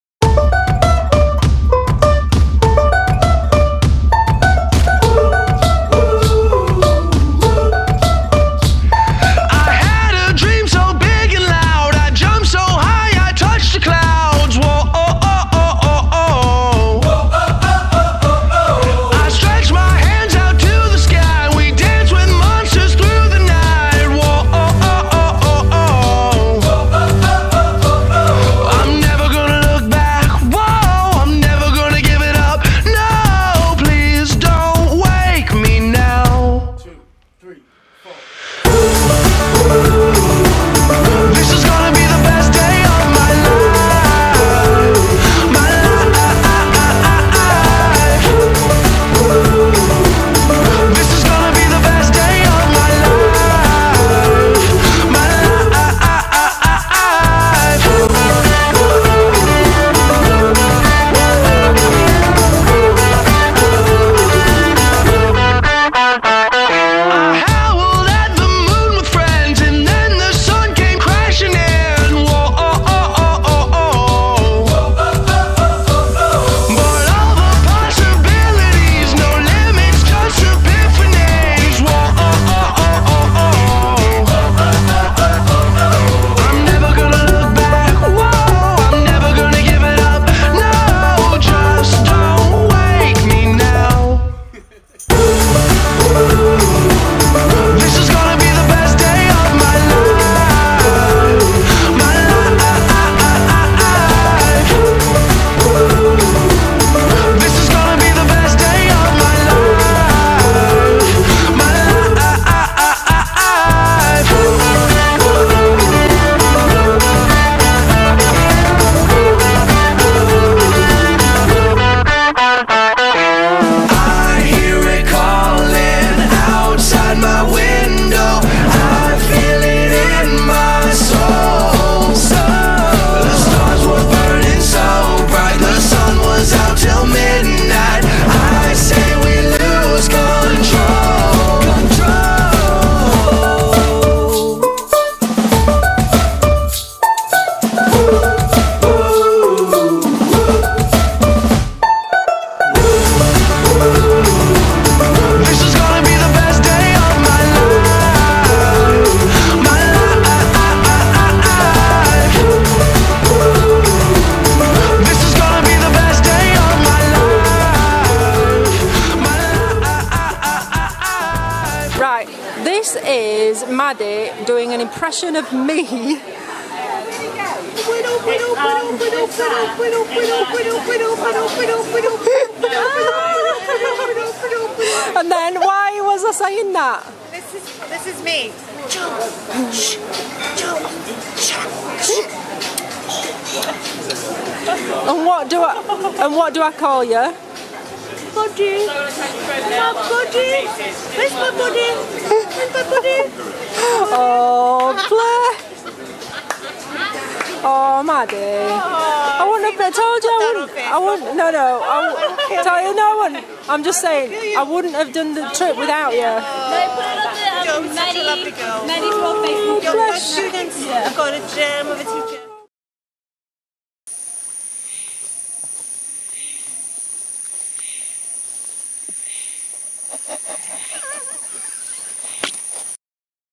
Borneo Buddies - August 2013
This is just a snippet of all the photos that I took during a 2 week trip to Borneo August 2013. Without my amazing "Borneo Buddies" it just would not have been the same.